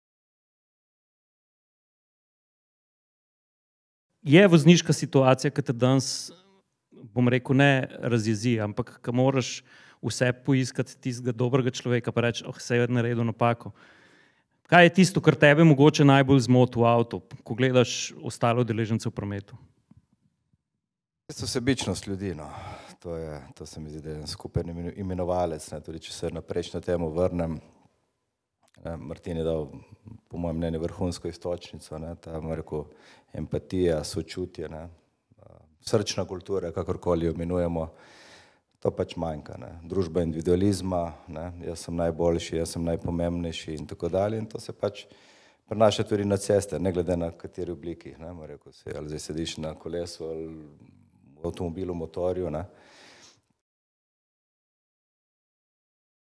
KAJ TE MOTI NA POTI? - Andrej Jereb 2
Na dogodku, ki ga je organiziral Continental, pod okriljem projekta DELIMO SI POT*, so vodenje pogovora prevzeli podcast mojstri Atmosferci, gostili pa smo tri strokovnjake iz različnih področij, ki jim je skupno to, da so udeleženci v prometu: Andreja Jereba, nekdanjega vrhunskega rally voznika, Martina Hvastijo, direktorja reprezentanc KZS in strokovnega komentatorja na RTV SLO in Vojka Safrana, AMZS inštruktorja varne vožnje.
Andrej Jereb o sebičnosti na cesti.
*Delimo si pot je Continentalova globalna akcija (angleško imenovana »Share the road«), ki javnost ozavešča o pomenu varnosti na cesti, sobivanju vseh udeležencev v prometu in poziva k strpnosti in previdnosti.
At an event organized by Continental under the auspices of the project SHARE THE ROAD*, the conversation was led by the podcast masters Atmosferci. We hosted three experts from different fields who share a common experience as participants in traffic: Andrej Jereb, a former top rally driver; Martin Hvastija, the director of national teams at the Cycling Federation of Slovenia and a professional commentator on RTV SLO; and Vojko Safran, safe driving instructor at Automobile and Motorcycle Association of Slovenia.
Andrej Jereb discusses selfishness on the road.
*Share the road is Continental's global initiative, which raises public awareness about the importance of road safety, coexistence of all traffic participants, and advocates for tolerance and caution.